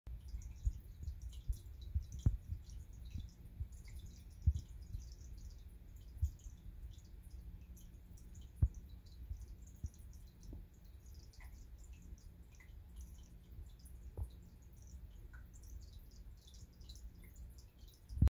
My Oto Catfish Is 'KISSING' the Ass of a Red Lizard Catfish
Tracking the scent of dried artemia eggs, both an oto catfish and a red lizard catfish looked through the bottom of my nano cube. Suddenly the oto approached to the red lizard and eagerly touched red lizard's ass with its mouth.

This peculiar motion is similar to so-called 'T-Position'; a unique position that a pair of mature corydoras conducts while mating. It is impossible for me to investigate the reason why my oto catfish performed such unusual behavior accurately.